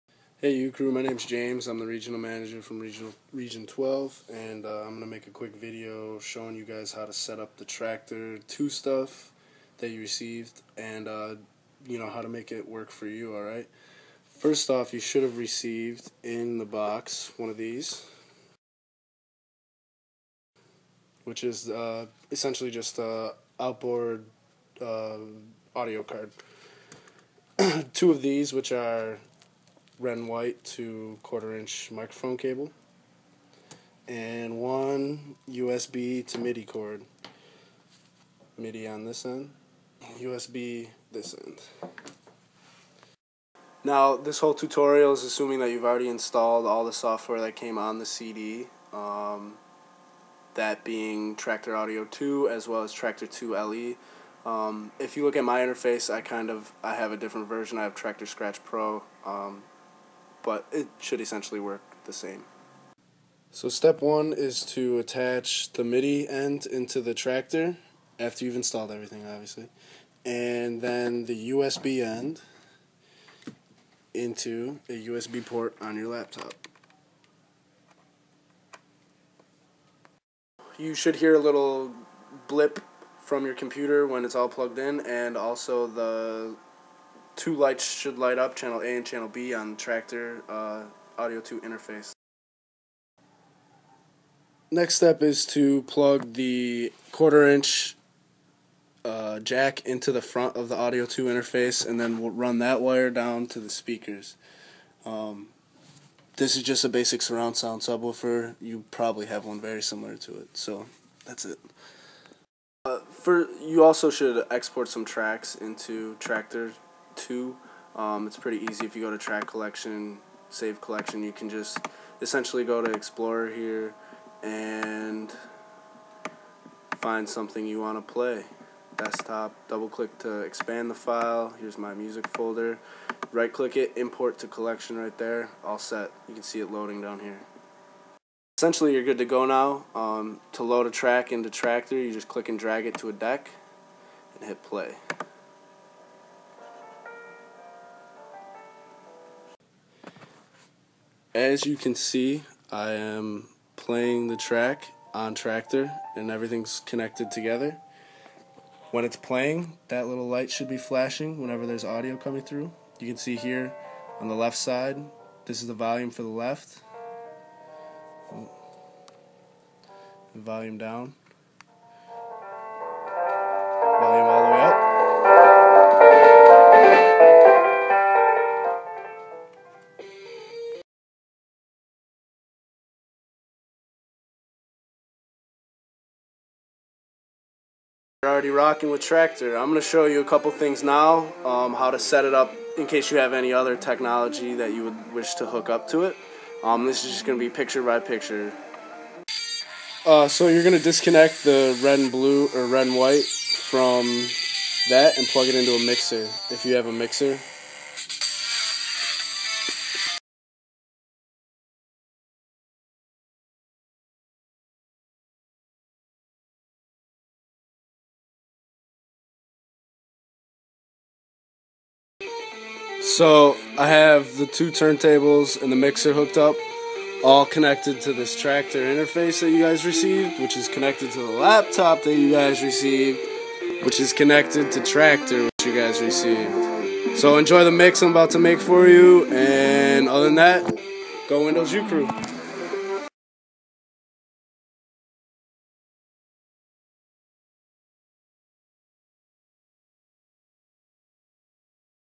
How to set up Traktor Audio 2 Interface
Quick video showing the basics of setting up the Traktor 2 Interface